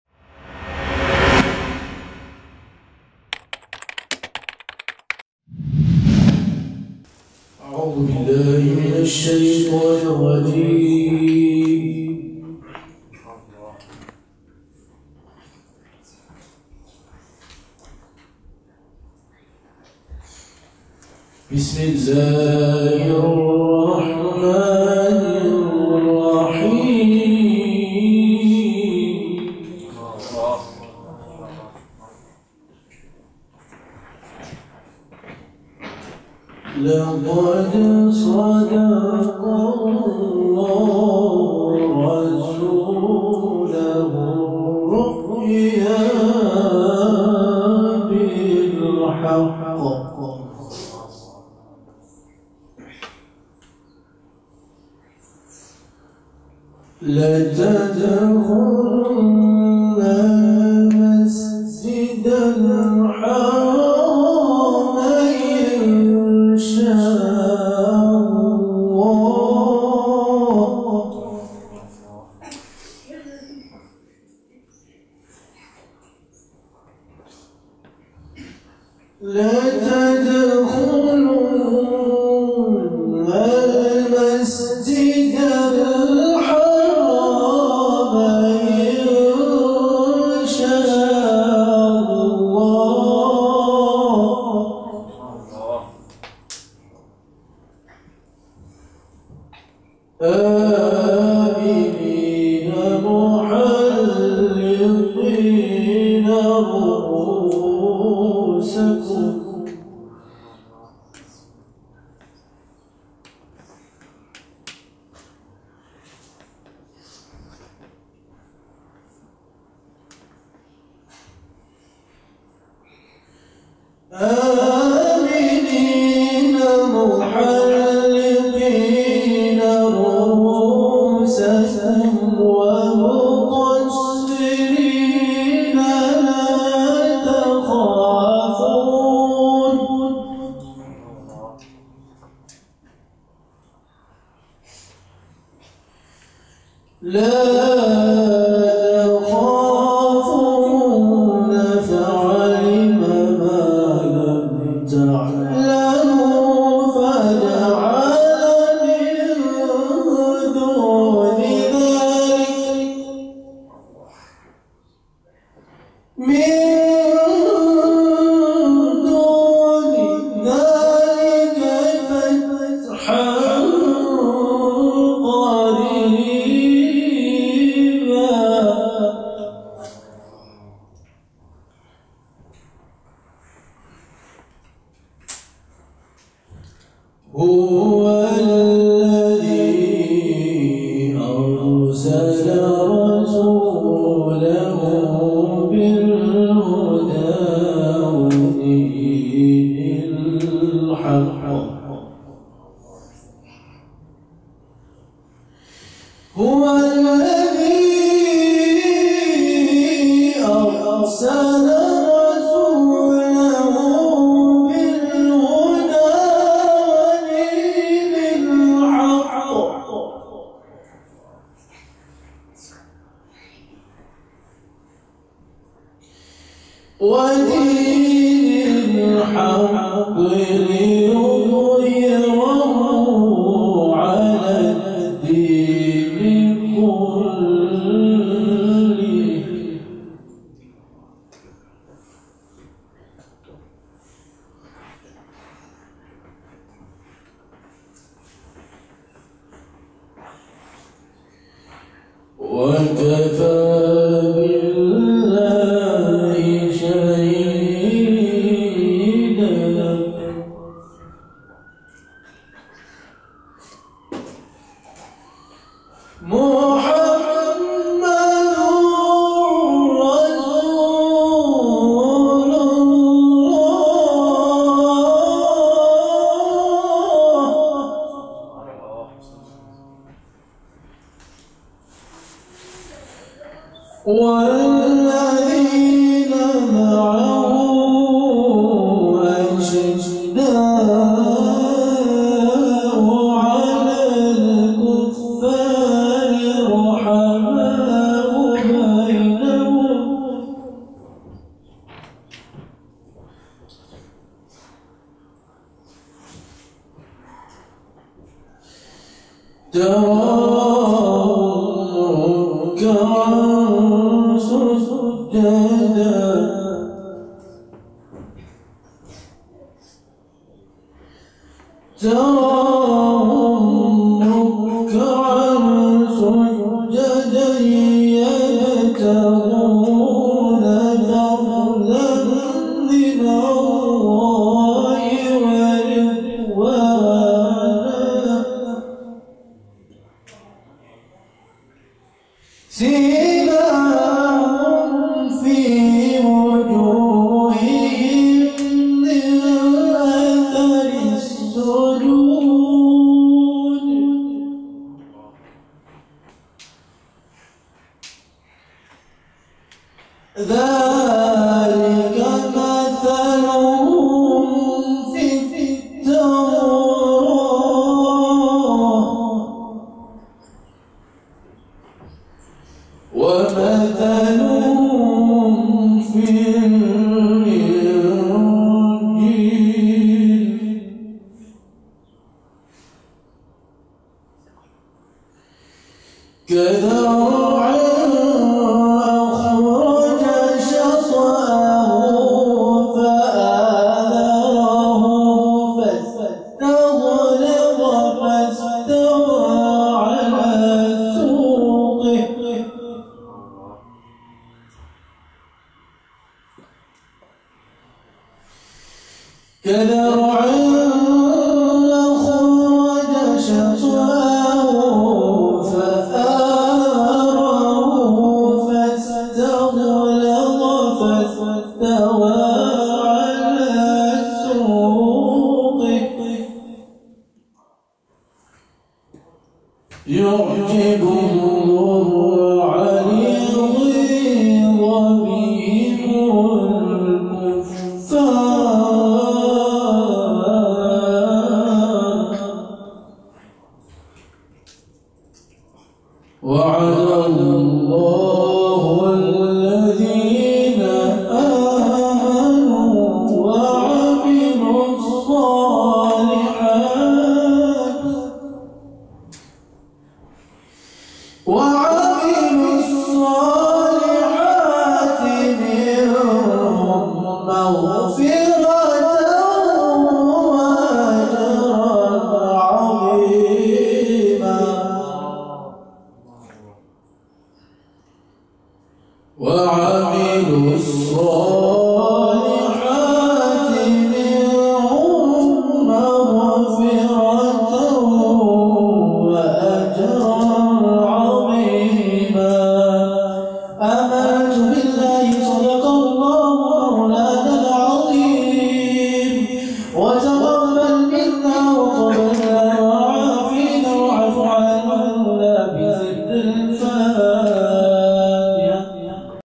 Quran Recitation | Night of Salawaats
Quran recitation by Brother Abdul Shakoor at the Pearls of Madina Gathering: Night of Salawaats in Reading on Saturday, February 8, 2014.